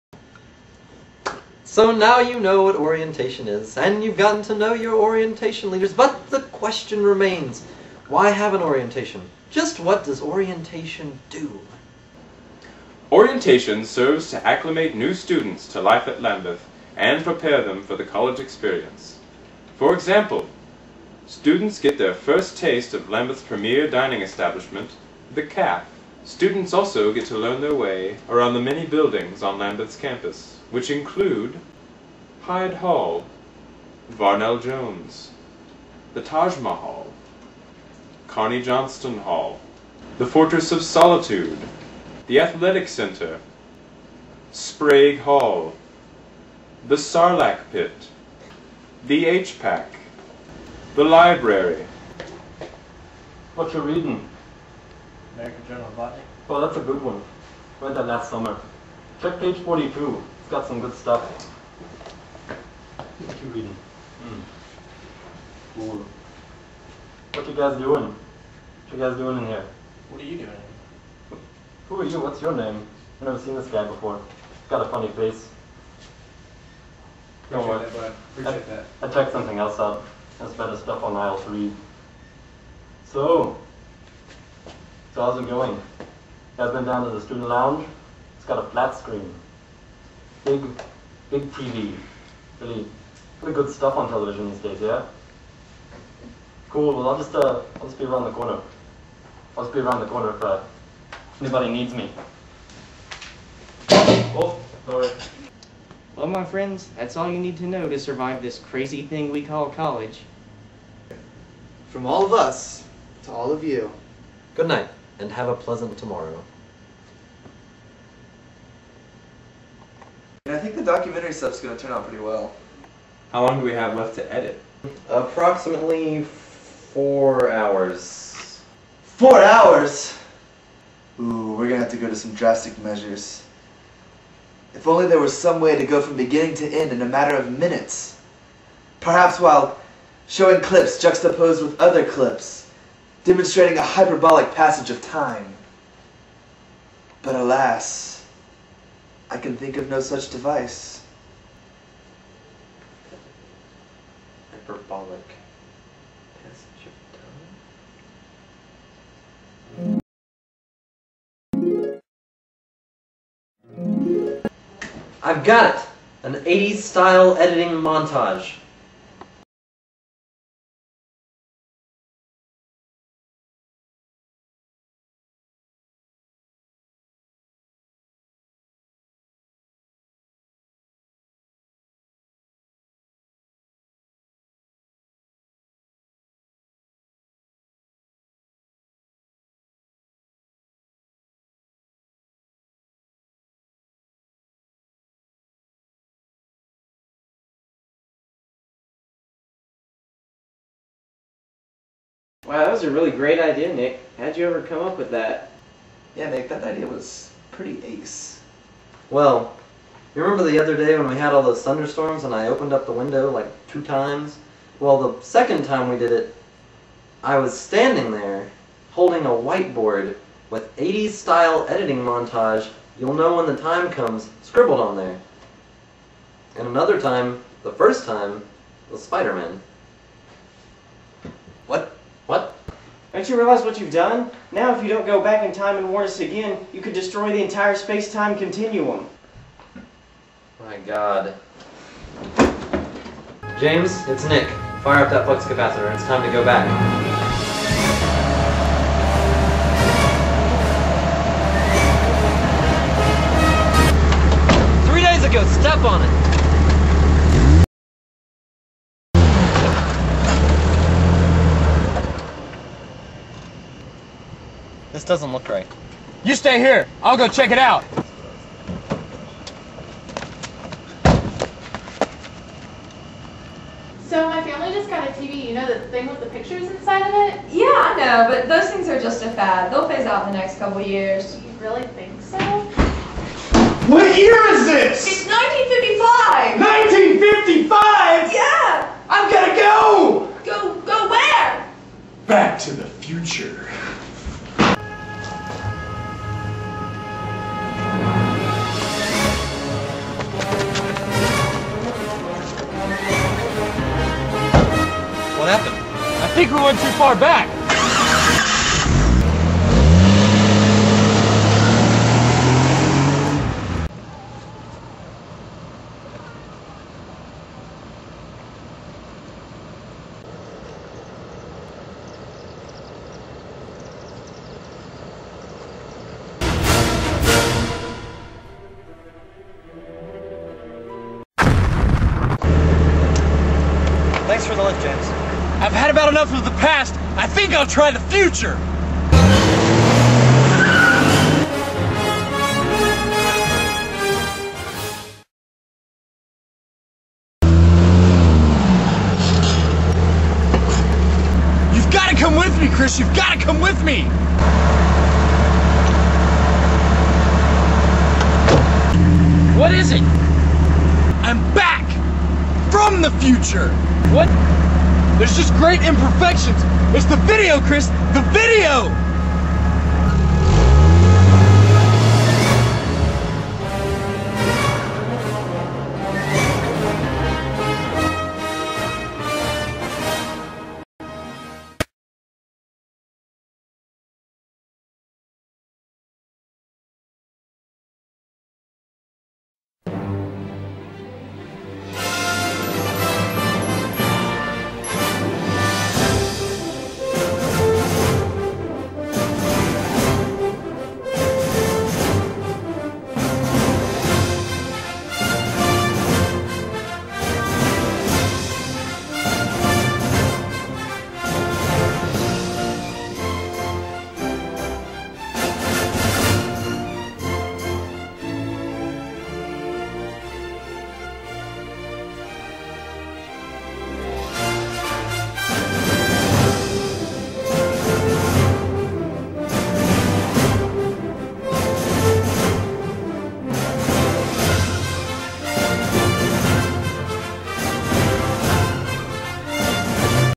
2008 Lambuth Orientation Video, Part 4 of 5
The 2008 installment of the Lambuth University New Student Orientation Video. Written and directed by Chris Bright and Nick Campbell. Starring Chris Bright, Nick Campbell, Matt Farmer, Justin Luzader, and the 2008 Lambuth Orientation Staff.